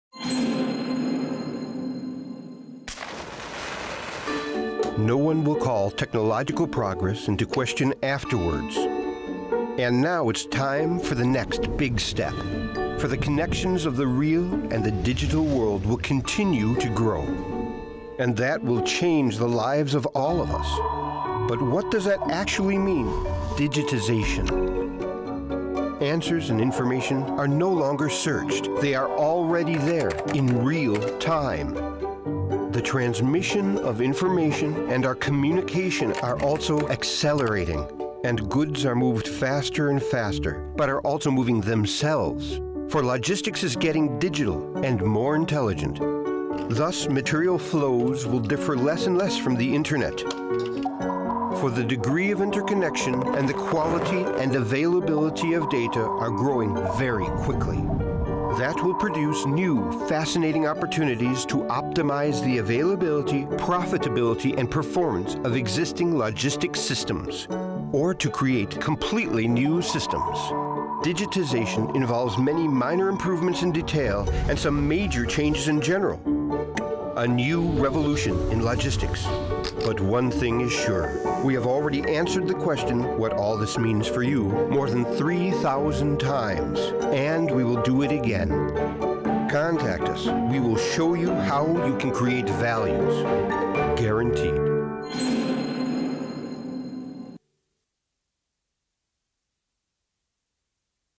Industry 4.0 - Intralogistics as a pioneer
The intelligent networking of production and value-added processes – generally known as Industry 4.0 – has long been more than just a vision. The linkages of real and digital worlds are growing, and that will produce new fascinating opportunities to optimize the availability, profitability and performance of existing logistics systems, or to create completely new systems – an industrial revolution. Intralogistics is a pioneer in this development and one of its most important technical pacemakers. Would you like to increase the added value of your company using Industry 4.0? viastore can show you how.

Examples from the real world: Intralogistics is the key to digitization and networking

In intralogistics, systems are controlled automatically and on the basis of data. This leads to reduced system dynamics at times of low demand, which involves less wear and less energy consumption. Moreover, the automated detection of frequently demanded product combinations reduces process times through route-optimized storage.
Moreover, the flexibility in production and distribution is increased through a holistic material flow management. For instance, parts for individual customer orders from different supply areas are consolidated shortly before order processing and delivered to the workstation just in time. This facilitates reduced warehousing and manufacture down to batch size 1. Furthermore, individual
order consolidation and refinement just before shipment saves storage space and reduces the risk of loss of unsold finished products.
The fast, proactive troubleshooting and analysis using performance and master data facilitate maximum delivery reliability and can minimize impending system shutdowns, which leads to a further increase in supply quality and delivery reliability.

Innovative examples from viastore:
viastore has developed a software which allows the controls of networked material flow systems to be commissioned digitally, and was awarded a position in the "100 Places for Industry 4.0" competition.